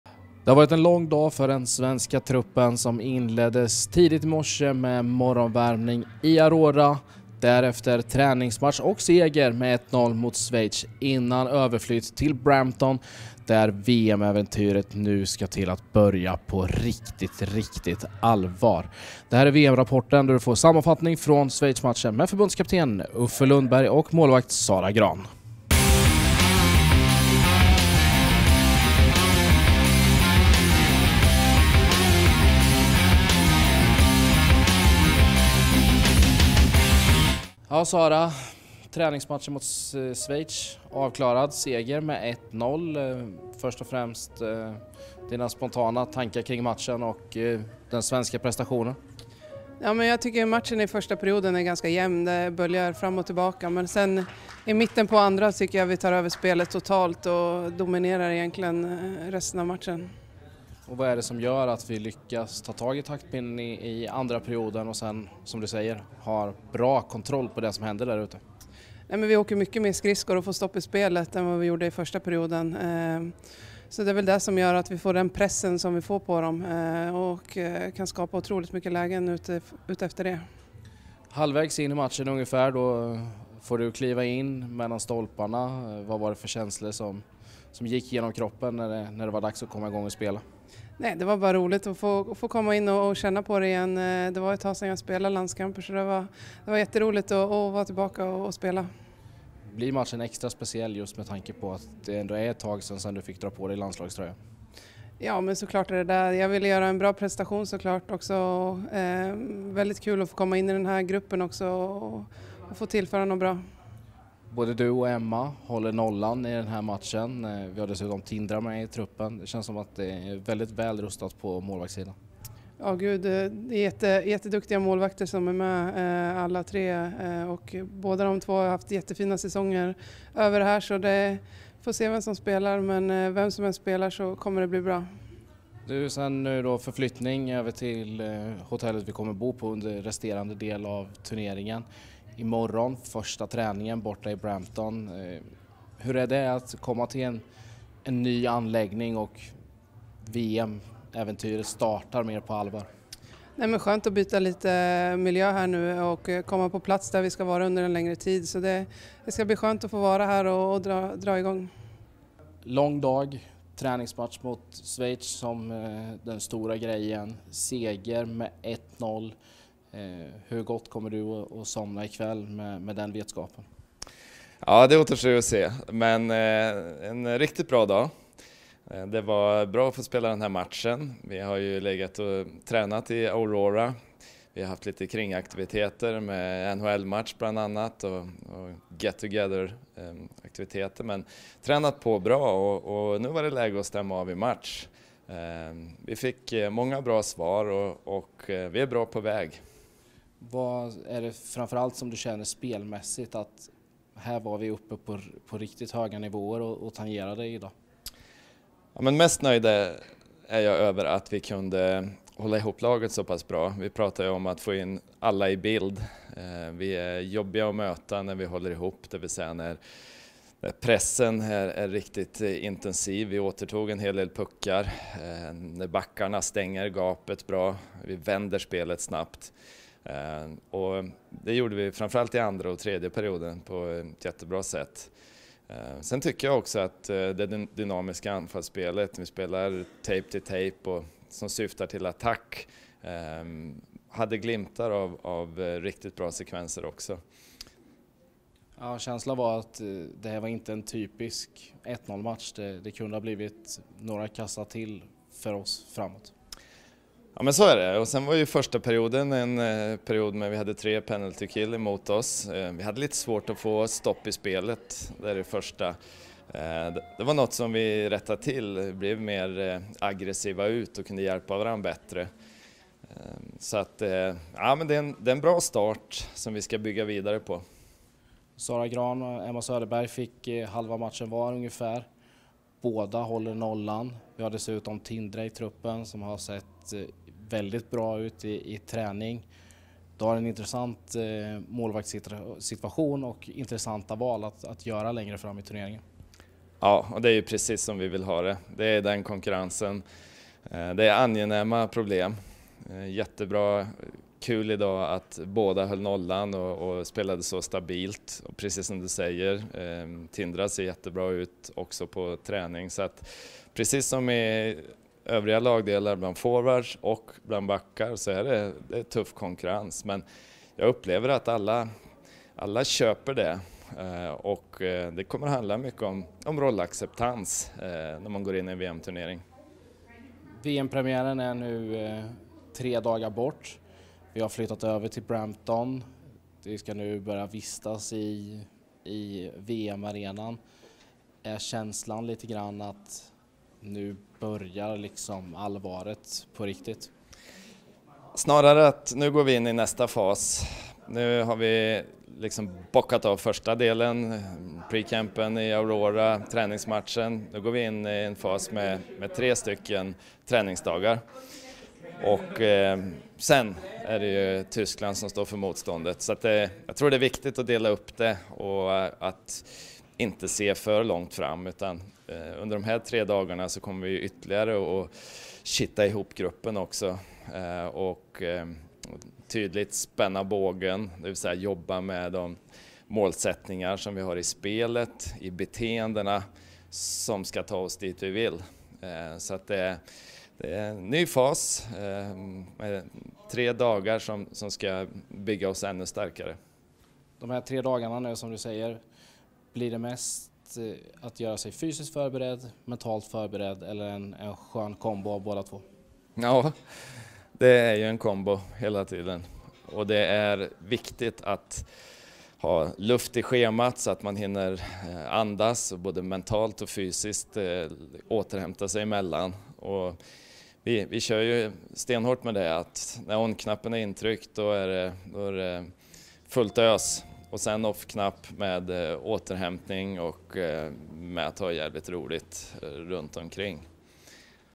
VM-Rapporten: Seger och flytt till VM-arenan
I dagens avsnitt av VM-Rapporten sammanfattar vi en dag som har varit lång och innehållit mycket. Höjdpunkten var träningsmatchen mot Schweiz, som slutade med en 1-0-seger. I programmet hör du Uffe Lundberg och Sara Grahn.